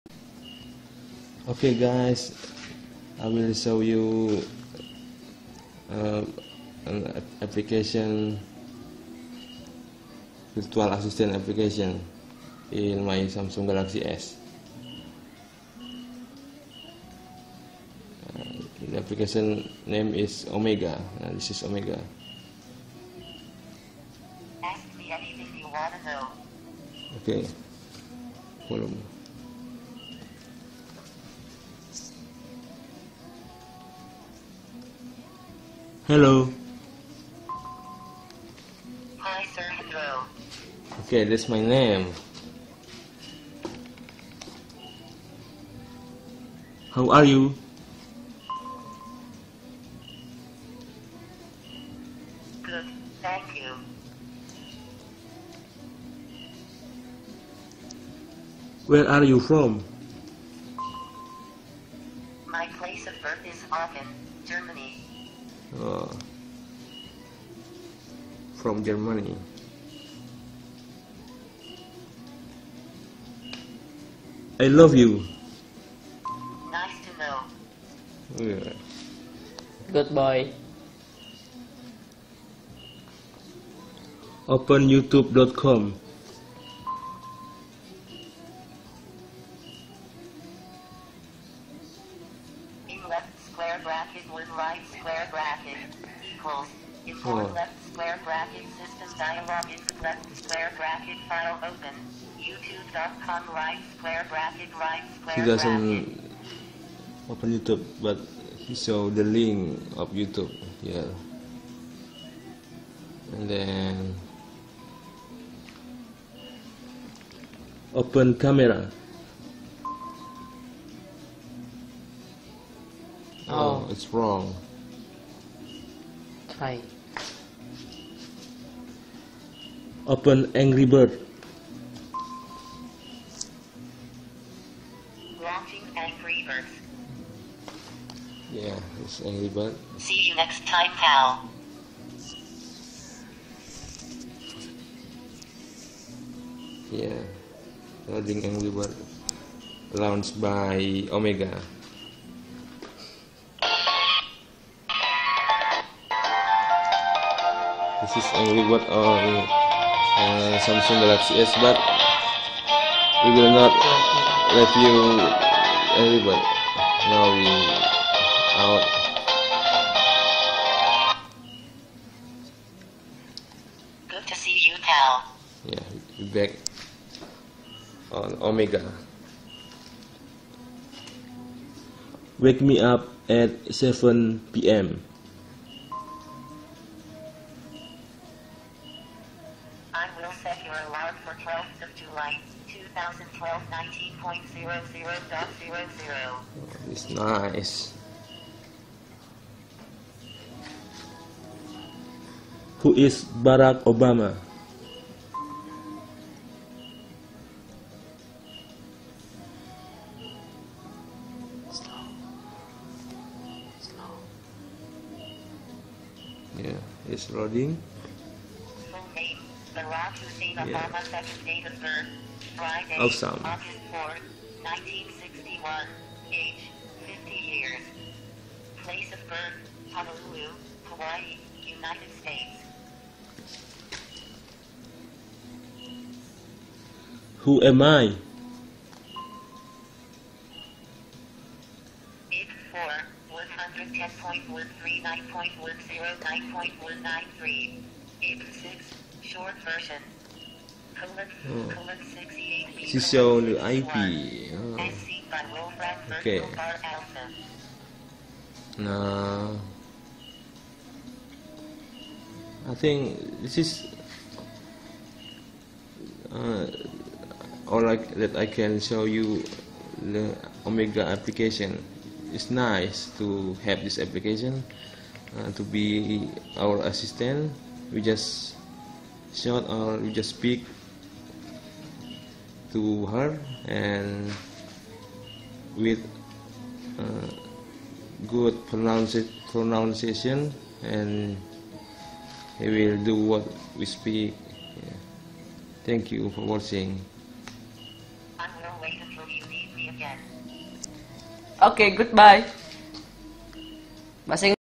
Omega for Android - The Virtual Assistant.mp4
If you can't install Siri or S-Voice. This aplication is recommended for you.
You have to connect to the internet for use it.
And you have to say the word right (good pronunciation).

Recorded by Samsung Galaxy Tab 7+
(Hendro feat. Reza)